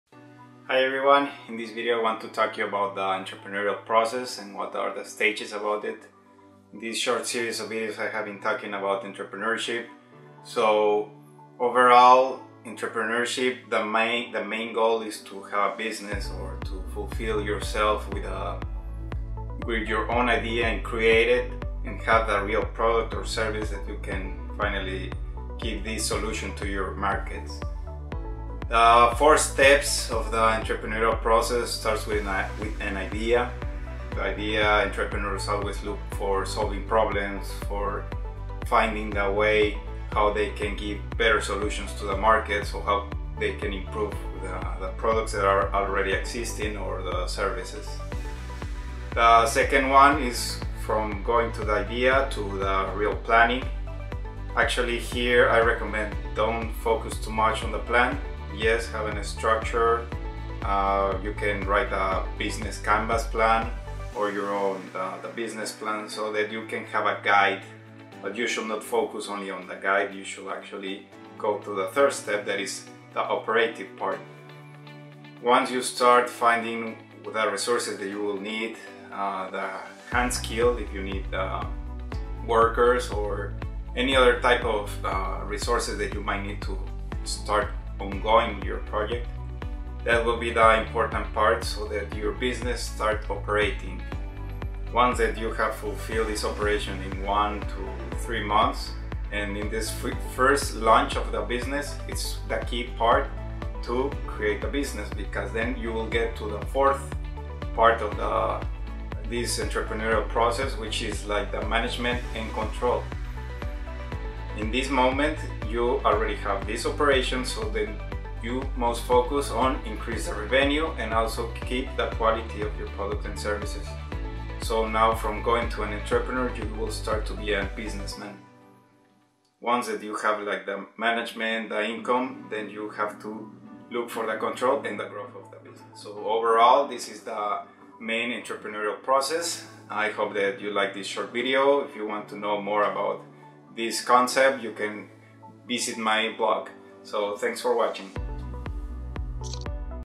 ⭐ Entrepreneurial Process
The entrepreneurial process stages that needs to happen to start a business is not easy for anyone. Many people have reasons to be an entrepreneur, many say the things that should be done, and a few are the ones who really do what is needed to start. It is very common to see people who think, but a few who act. The entrepreneurial process stages can be developed one step at a time, but some entrepreneurs are the ones who continue on this lifestyle.

The entrepreneurial culture and spirit is on those who decide to take one step ahead to achieve success. It is a long-term process, that visionaires will have to keep on working to transform their environment.